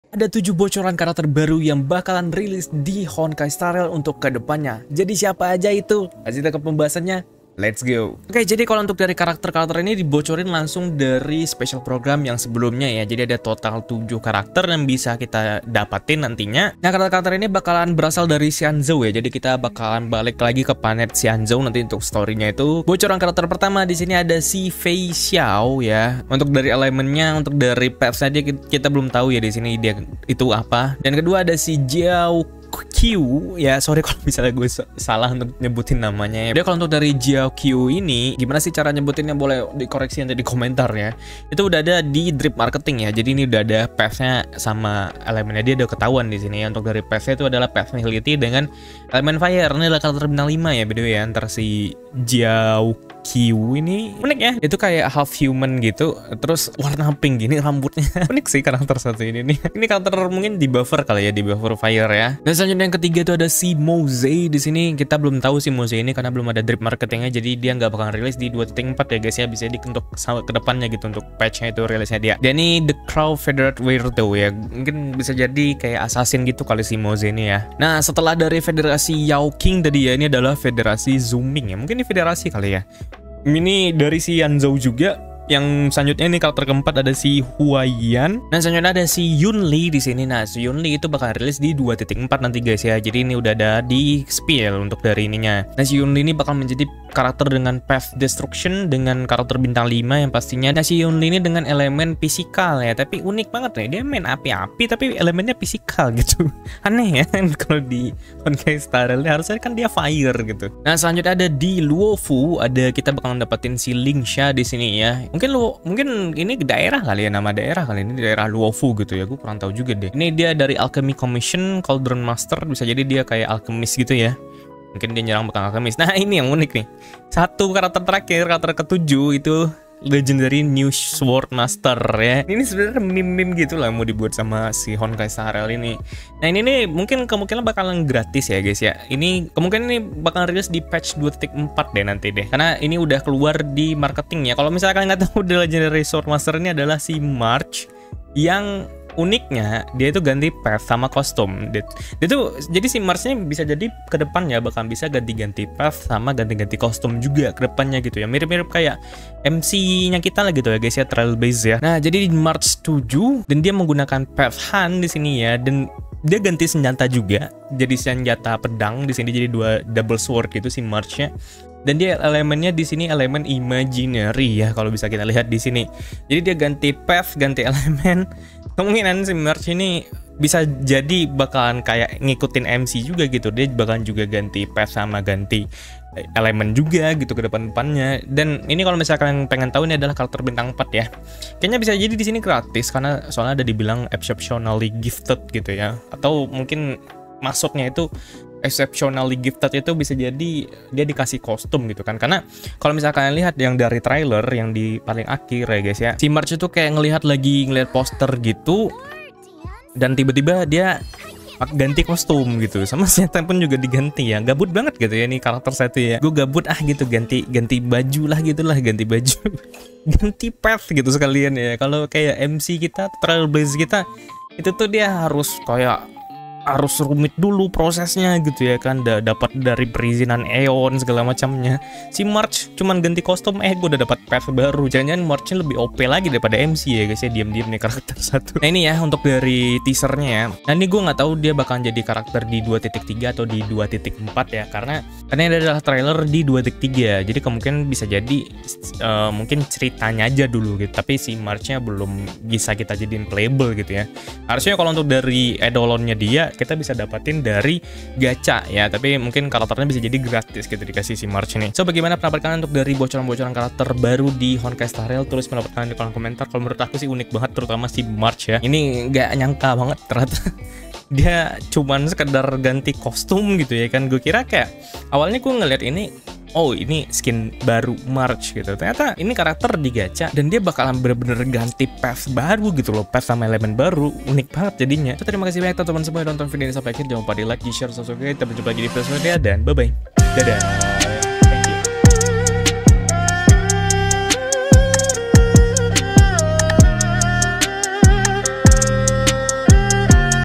March Dapat Path Baru 7 Bocoran Karakter Baru di Honkai: Star Rail!
7 Bocoran Resmi Karakter Baru yang Akan Segera Rilis di Honkai: Star Rail!

Halo semuanya! Di video kali ini, kita akan membahas 7 bocoran resmi tentang karakter baru yang akan segera rilis di Honkai: Star Rail. Jangan sampai ketinggalan informasi terbaru tentang game favorit kita ini!

Langganan Membership Channel: Join Membership
Join Discord Isra: Join Discord
Follow Instagram: Follow Instagram